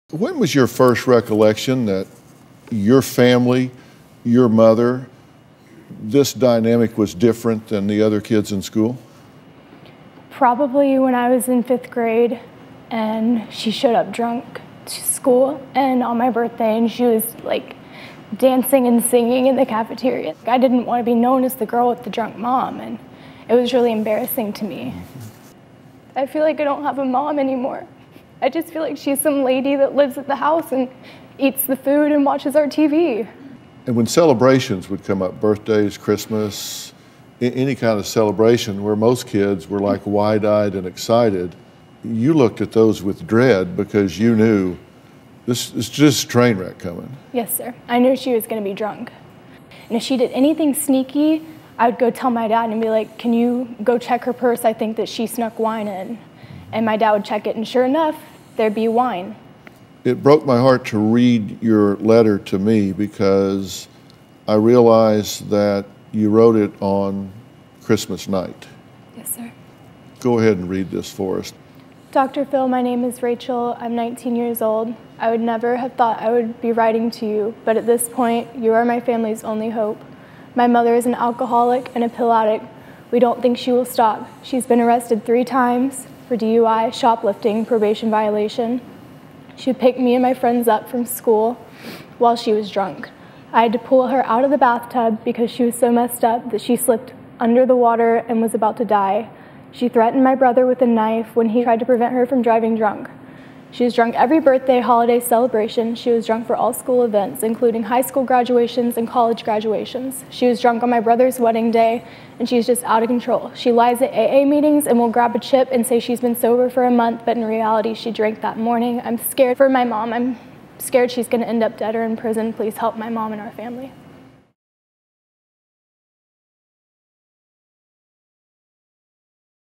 Why Teen Says In Elementary School She Became Known As ‘The Girl With The Drunk Mom’
A teen describes an incident when she was in fifth grade where her mom showed up at her school drunk and how it has affected her all these years.